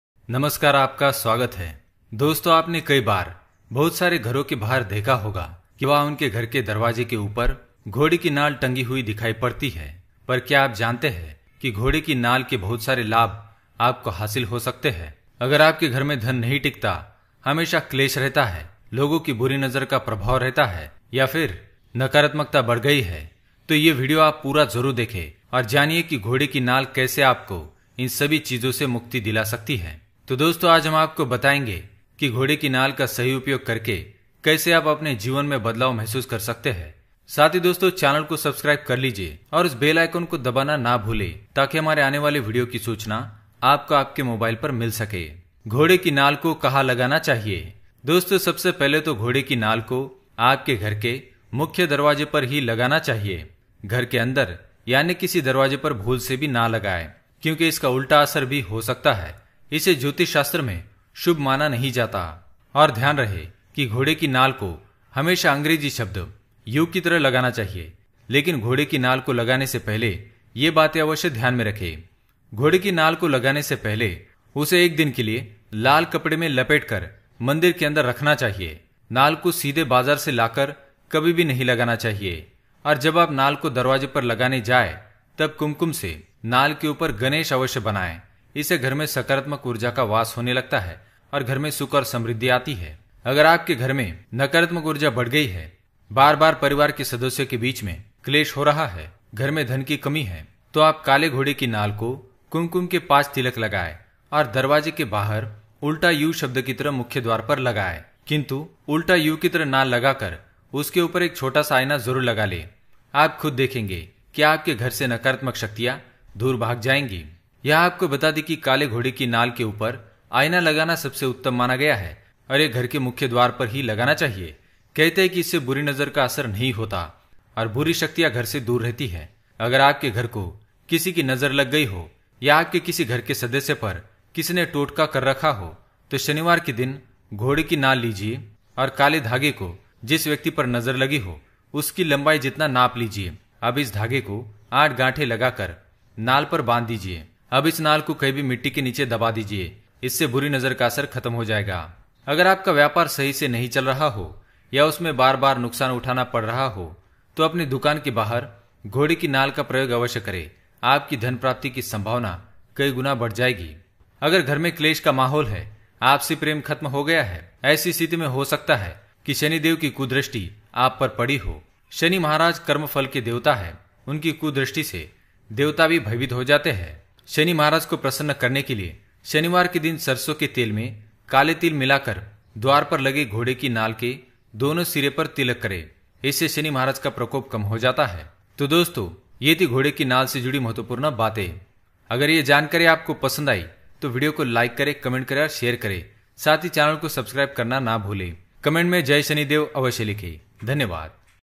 घोड़े की नाल को घर में इस जगह पर ना लगाए | कैसे करें घोड़े की नाल का प्रयोग How to use horse shoes
HOW TO USE HORSE NAIL SHOES